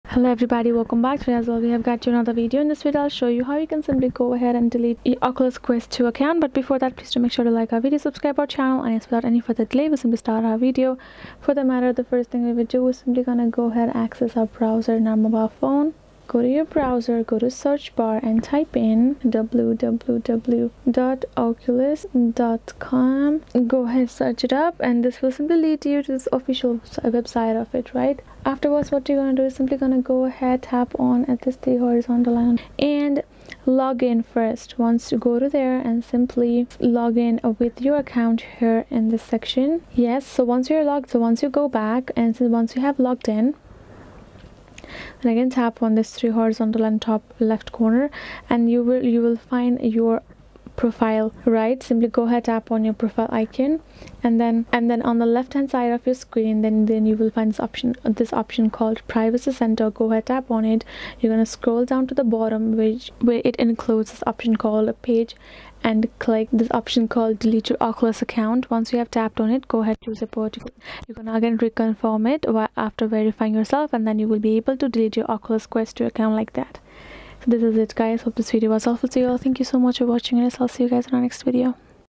How to Delete Oculus Quest 2 Account !
In this tutorial, I will guide you on how you can easily delete oculus quest 2 account.
0:00 Intro
0:14 Access oculus.com
0:24 Delete oculus 2 account
1:17 Conclusion